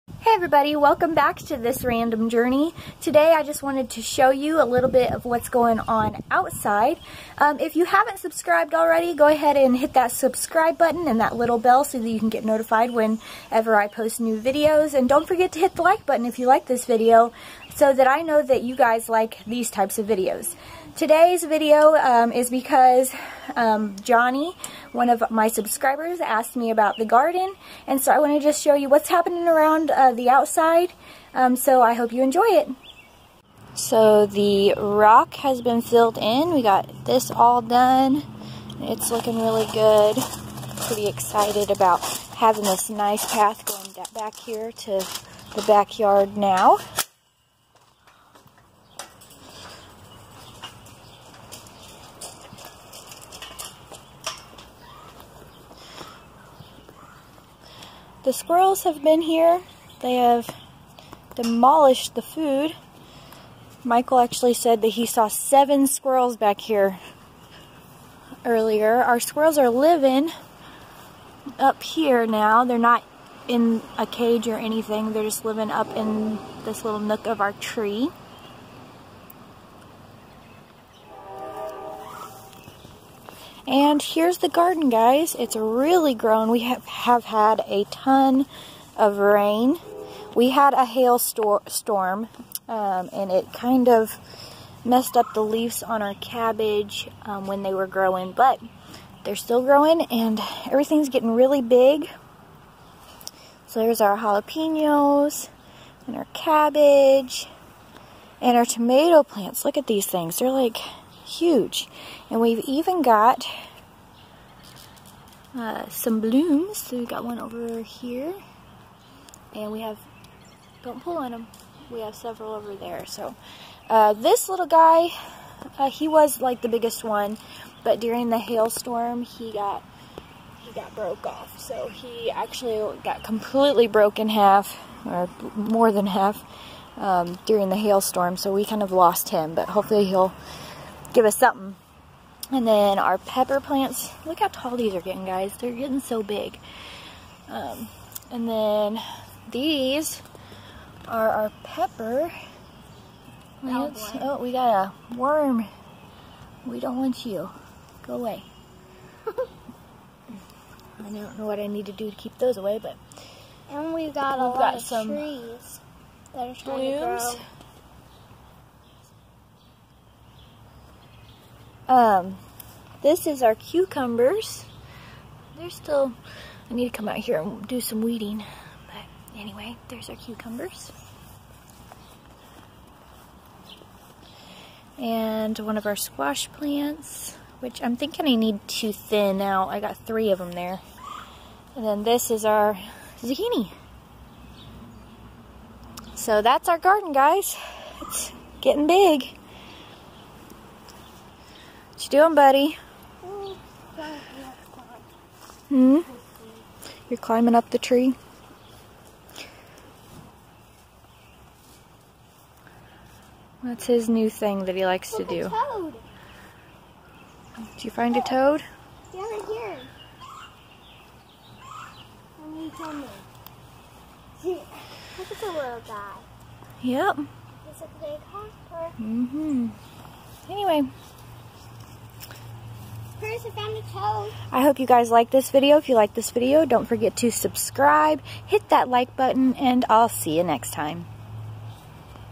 Garden Update
Our garden is growing so fast. Check out how much it's grown since the last time and see the path that's now completed.

Follow our Facebook Page, This Random Journey